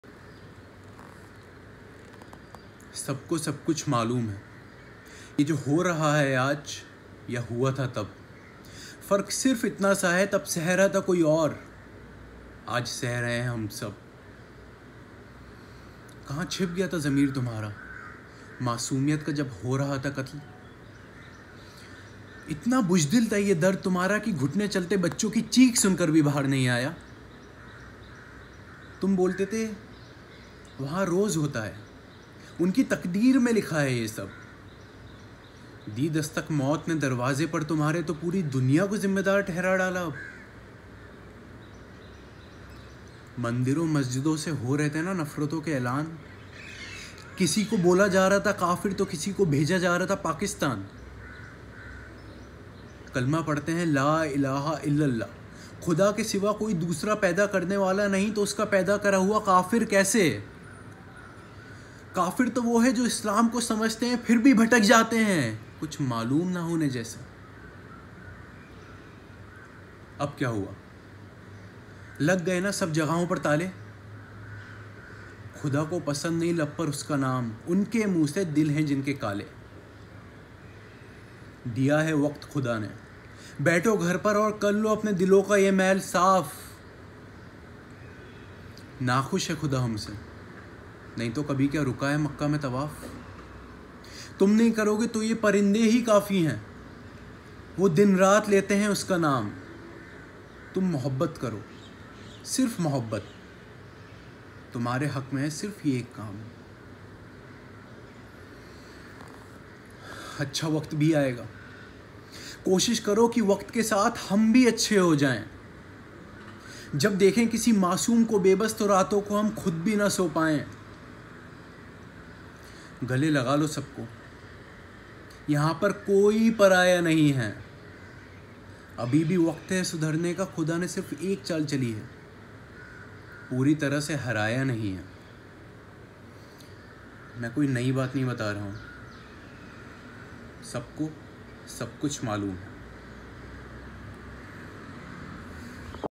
Sabko Sab Kuch Maloom Hai
I wrote a few lines this morning, please listen - “Sabko Sab Kuch Maloom Hai”